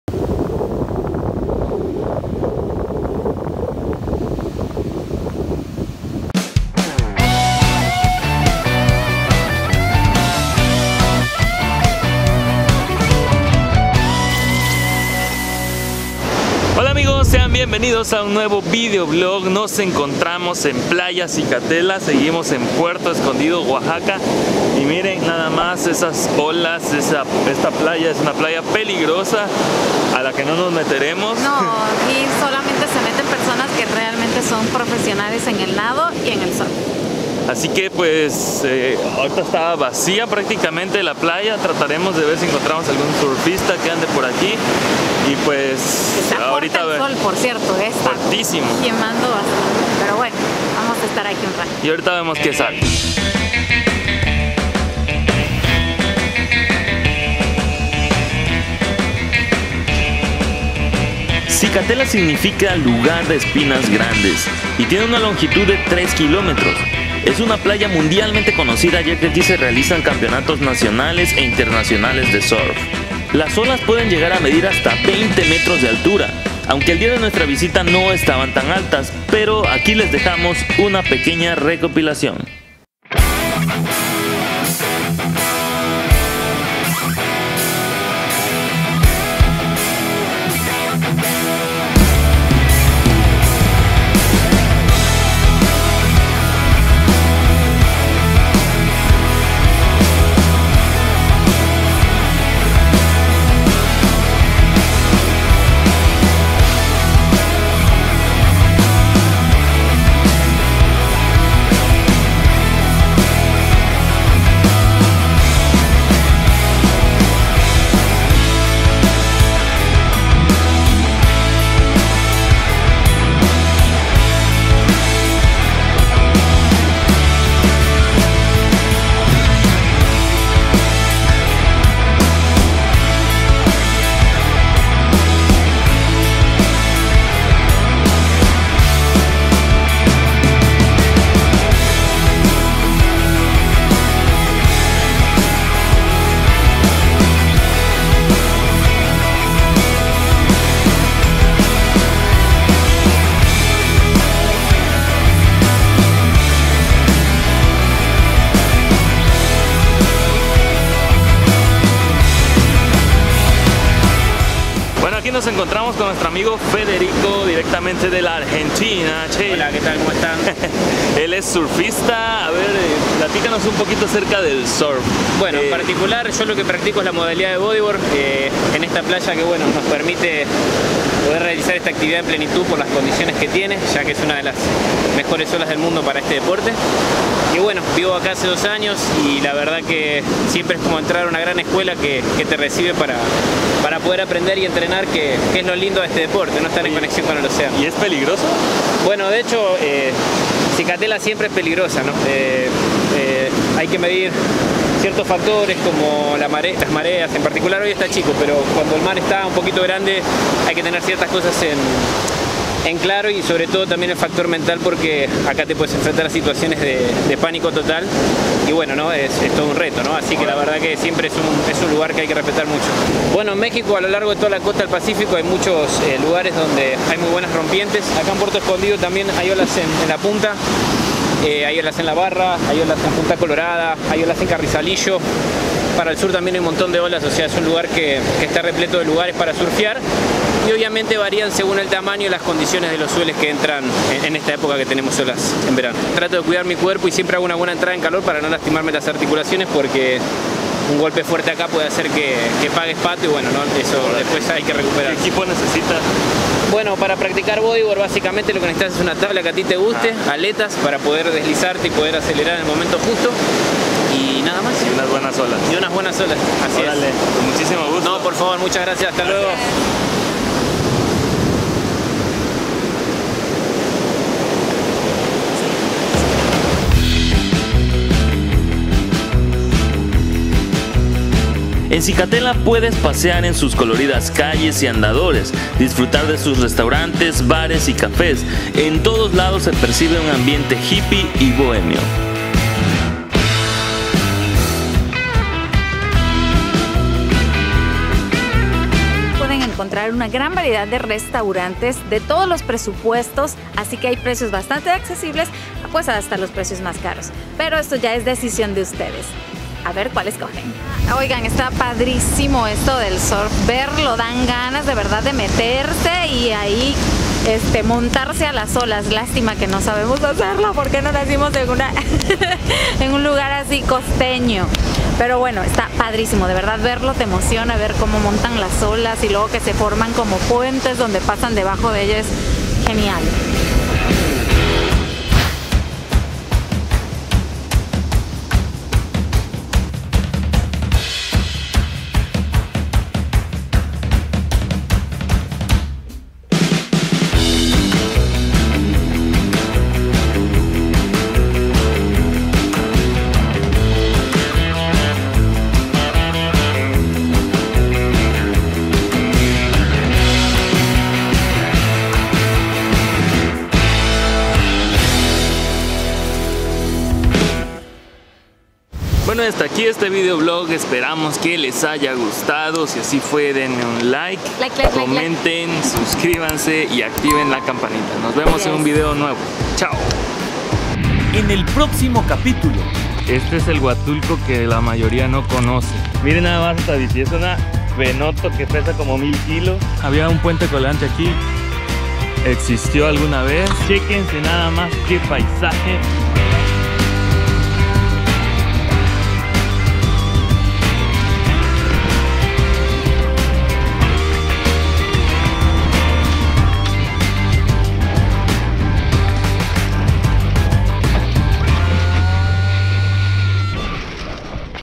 Playa ZICATELA | Puerto Escondido
Visitamos una de las playas más famosas y peligrosas de Puerto Escondido, Oaxaca... Playa Zicatela

Titular: Horacio Sánchez Olmos